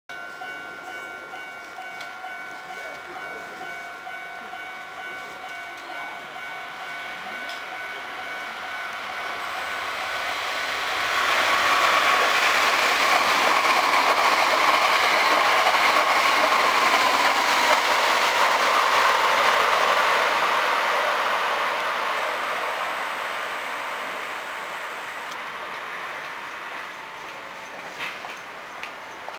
2019.03.16 阪急 5300系5301F 準急梅田行き通過 富田駅 阪急電鉄 阪急電車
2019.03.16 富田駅
5301F 7両編成
(Mc)5301-(M')5801-(T)5851-(T)5861-(T)5881-(M)5901-(M'c)5401